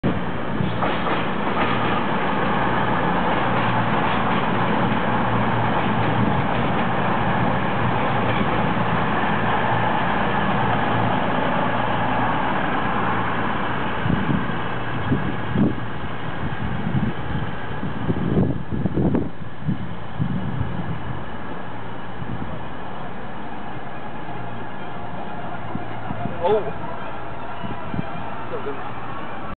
An evening at Doncaster (pt11)-a grab shot
In this quick grab shot we see a X-country class 220 Voyager call at Doncaster on it's way south.More to follow.